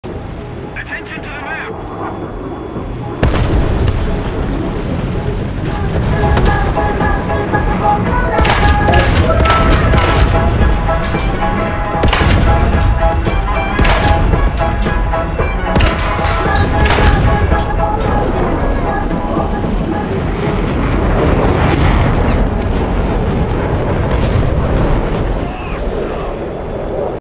Helping kill a Maus
Ystervark thought he had me.

Featured vehicles:
PT-76-57
Maus